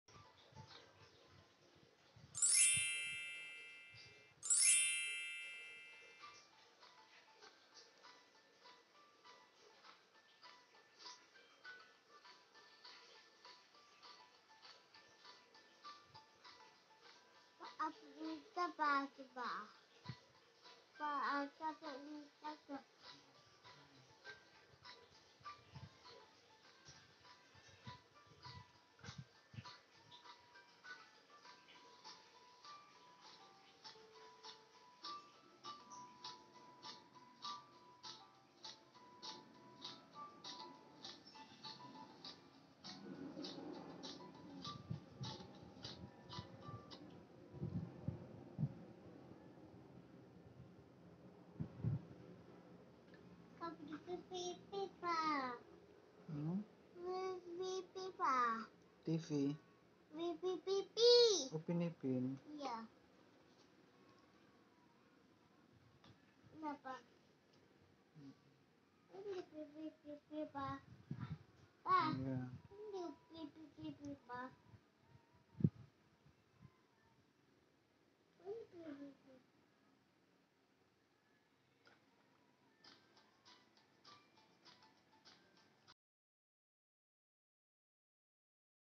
mewarnai jamur dengan kuas ajaib
belajar mewarnai dengan kuas ajaib mewarnai hewan
mewarnai tumbuhan ,mewarnai hewan laut dan masih banyak lagi